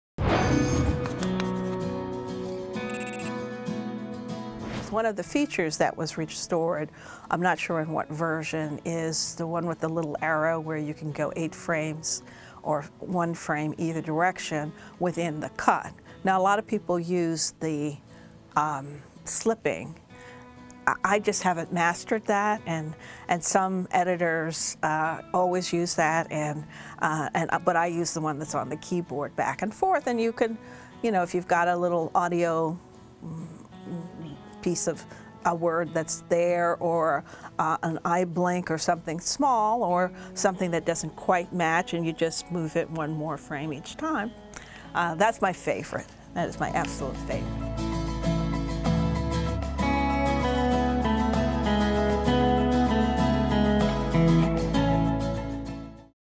The A-List — Lillian E. Benson, ACE — Favorite Media Composer Feature
Lillian Benson is known for her work on Maya Angelou and Still I Rise (2016), Chicago Med (2015), Life Is Not a Fairytale: The Fantasia Barrino Story (2006) and The Revolutionary War (1995). She was the first African-American female editor selected for membership in American Cinema Editors (ACE), and is a member of the board of directors of ACE.